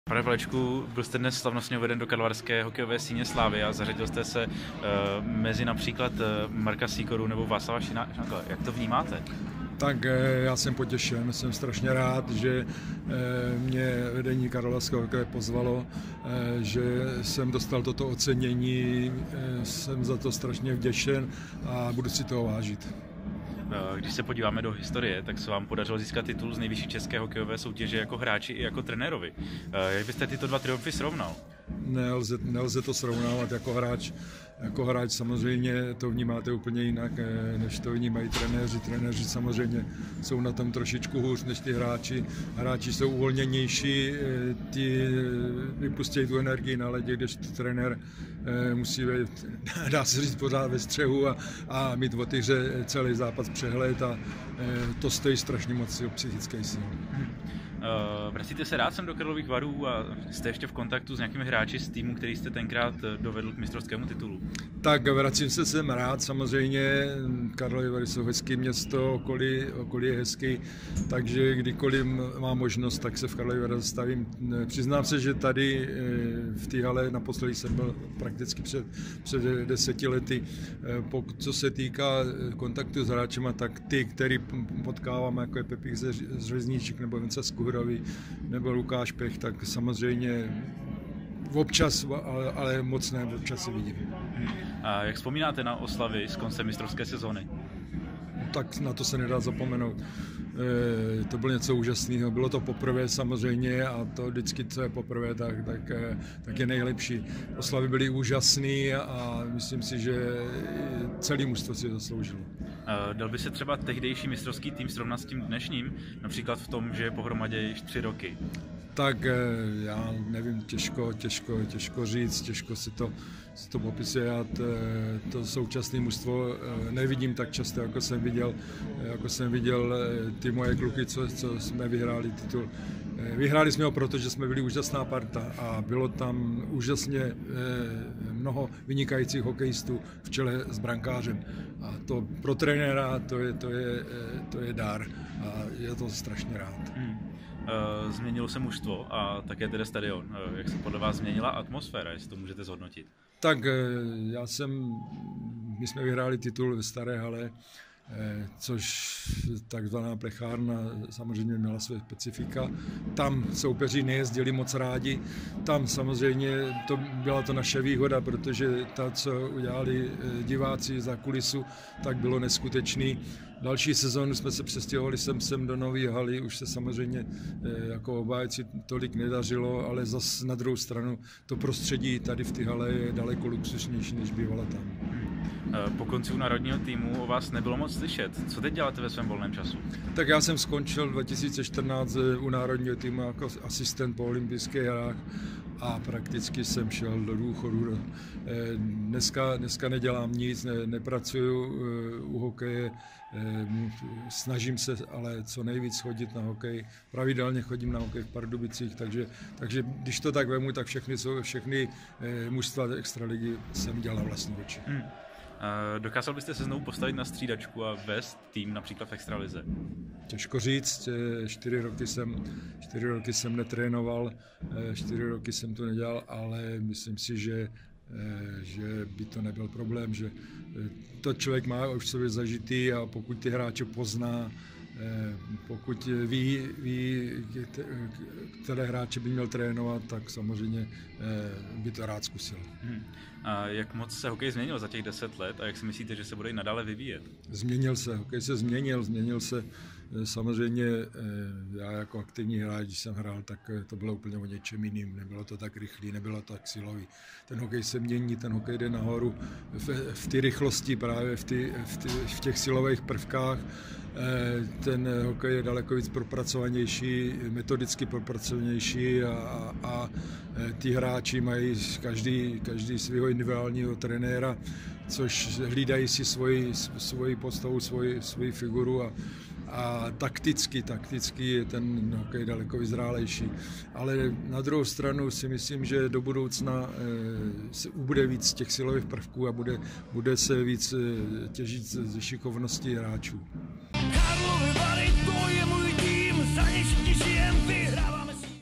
Byli jsme skvělá parta, vzpomíná trenér mistrů Paleček
Pan Josef Paleček, trenér, kterému se podařilo dovést Energii k zatím jedinému titulu. Člověk, který získal mistrovský pohár jako hráč i jako trenér. V úterý se po dlouhých letech vrátil do Karlových Varů, aby byl slavnostně přijat do síně slávy karlovarského hokeje. „Jsem strašně rád, že mě vedení karlovarského hokeje pozvalo a že jsem dostal toto ocenění. Jsem za to vděčen a budu si toho vážit,“ hodnotí trenér zlatého týmu Energie ze sezony 2008/09.